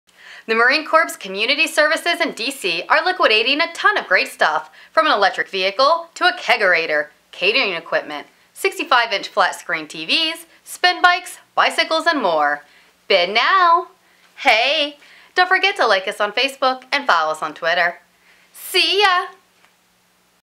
Marine Corps Community Services Online Auction
The Marine Corps Community Services in DC are liquidating a ton of great stuff from an electric vehicle to a kegerator, catering equipment, 65" flat screen TVs, spin bikes, bicycles, and more. Bid now!